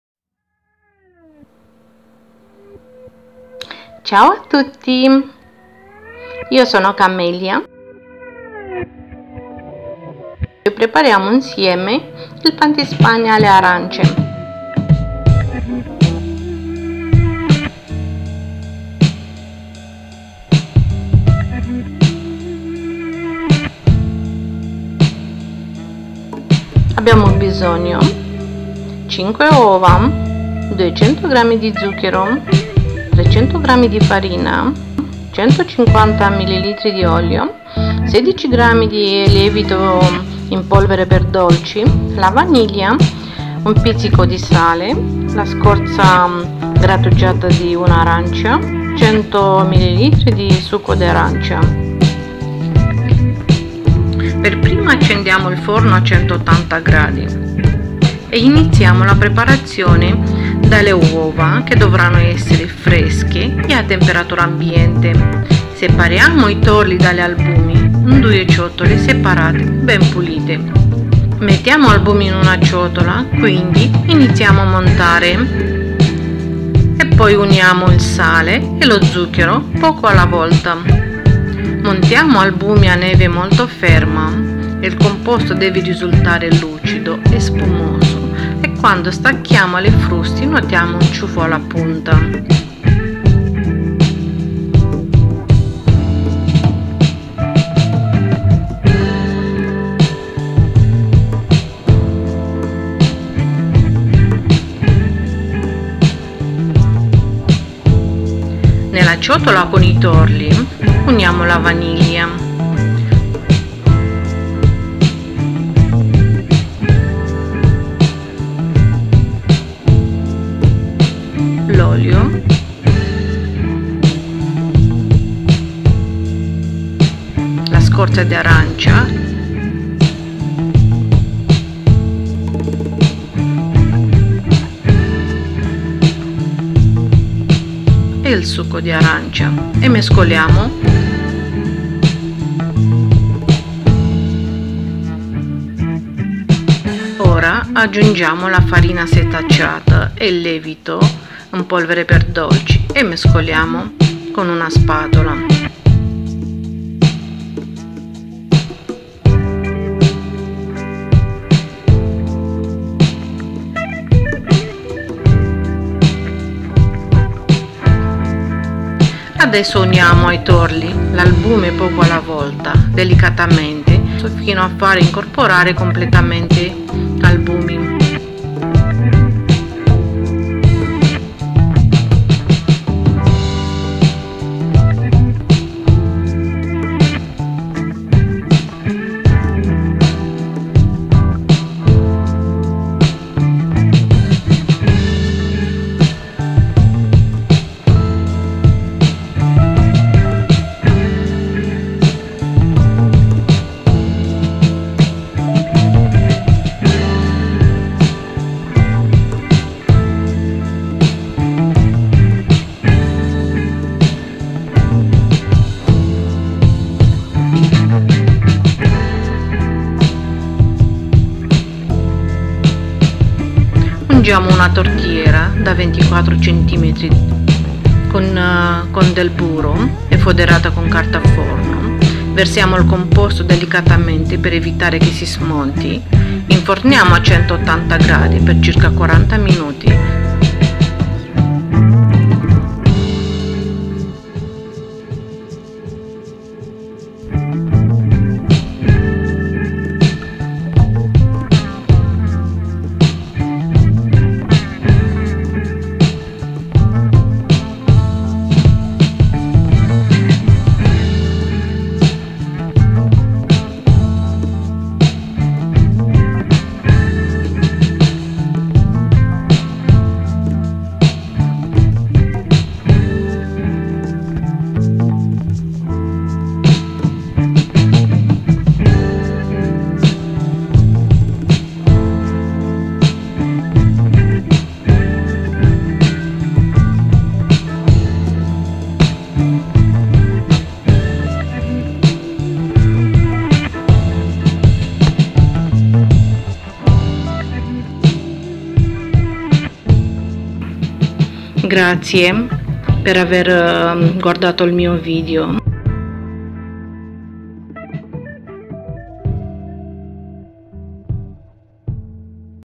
Pan di Spagna all'arancia / Blat de tort cu gust de portocale /Sponge cake with oranges / Kammellia
Ingrediente: / Ingredienti: / Ingredients:5 uova, 5 ouă, 5 eggs
200 g zucchero
200 g zahăr
200 g sugar

300 g farina
300 g făină
300 g flour

100 ml olio
100 ml ulei
100 ml oil

16 g lievito per dolci
16 g praf de copt
16 g baking powder

q.b. vaniglia
q.s. vanilie
q.s. vanilla

1 pizzico di sale
1 praf de sare
1 pinch of salt

buccia d'arancia
coaja rasa de portocala
orange peel

100 ml di succo d'arancia
100 ml suc de portocale
100 ml of orange juice

Va multumesc!